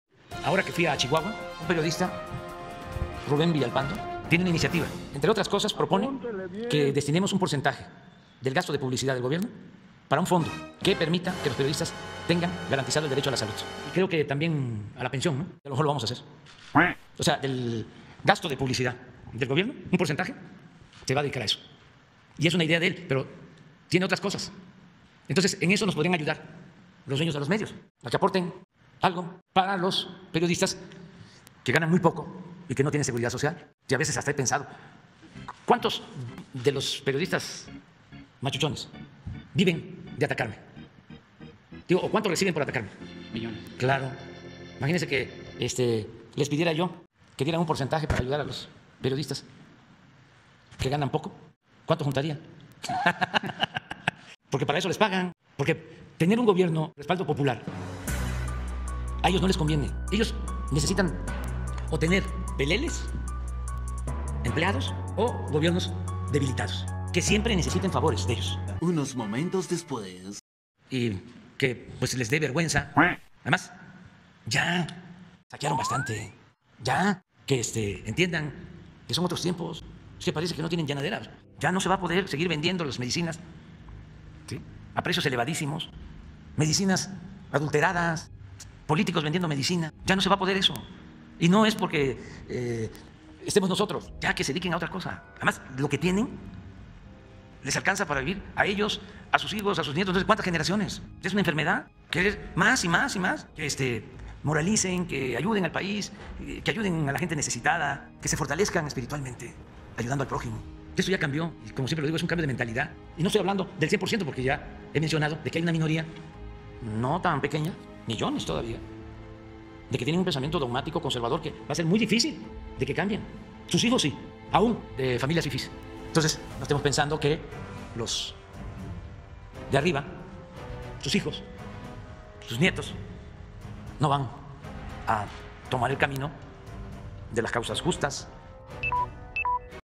Recibió una propuesta para crear un fondo para los periodistas que ganan poco 🦆
Rubén Villalpando me hizo la propuesta de que destinemos un porcentaje del gasto de publicidad para un fondo de salud y pensiones para los periodistas, “tal vez los dueños de los medios nos podrían ayudar a aportar para fortalecerse moralmente” 🦆